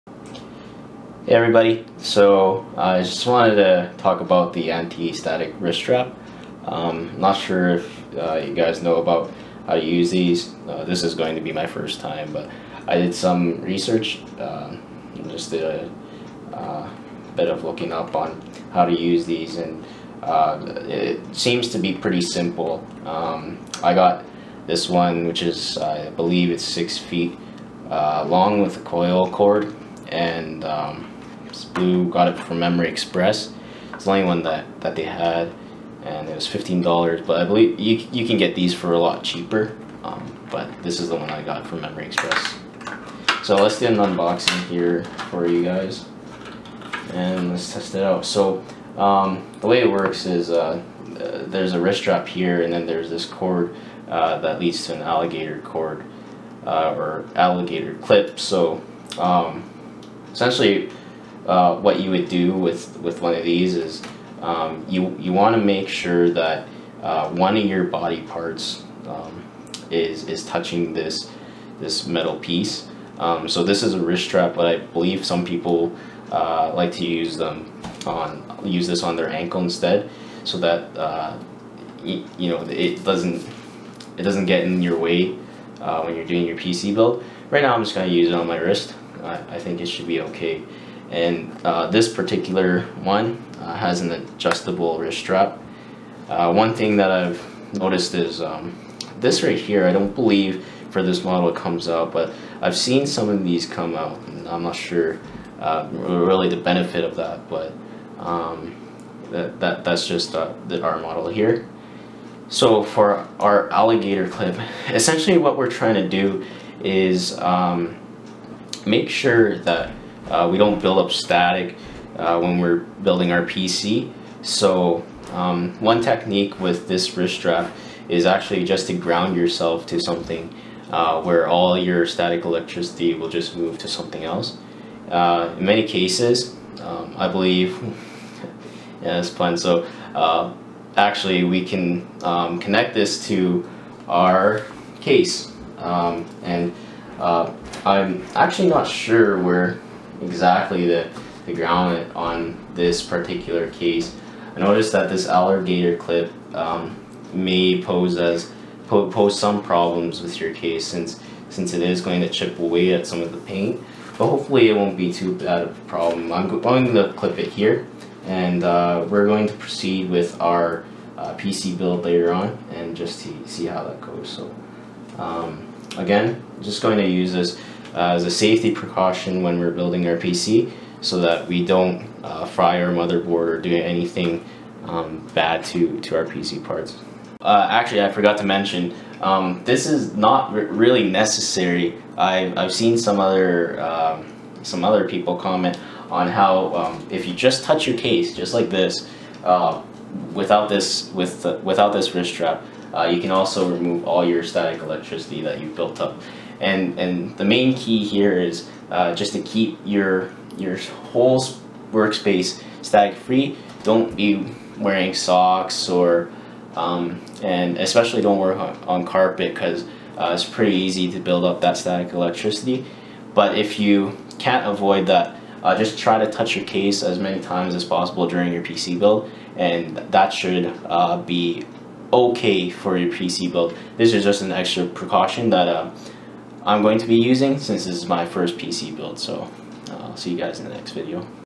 How to use antistatic wrist strap
Short clip on using the anti-static wrist strap as part of the DIY Gaming PC Build series.